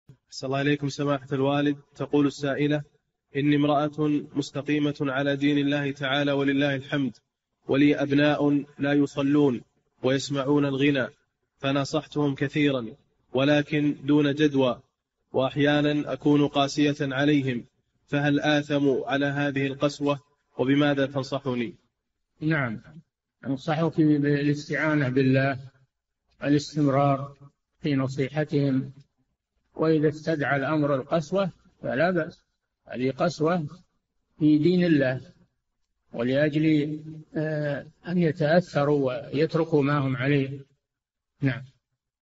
[2402- 4717] لي أولاد لا يصلون ويسمعون الغناء فنصحتهم ولكن دون جدوى فهل علي إثم إن قسوت عليهم؟
[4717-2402] أحسن الله إليكم سماحة الوالد، تقول السائلة: إني امرأة مستقيمة على دين الله تعالى -ولله الحمد- ولي أبناء لا يصلون ويسمعون الغناء فنصحتهم كثيرا ولكن دون جدوى وأحيانا أكون قاسية عليهم فهل آثم على هذه القسوة ؟ وبماذا تنصحني؟
الجَوَابُ: أنصحك بالاستعانة بالله، والاستمرار في نصيحتهم، وإذا استدعى الأمر القسوة فلا بأس هذه قسوة في دين الله، ولأجل أن يتأثروا ويتركوا ما هم عليه.
السيرة الذاتية :
للاشتراك في قناة الشيخ صالح الفوزان
لمشاهدة حلقات فتاوى على الهواء كاملة

للاستفسار والإقتراحات والدعم
رئيس الفريق
لمشروع كبار العلماء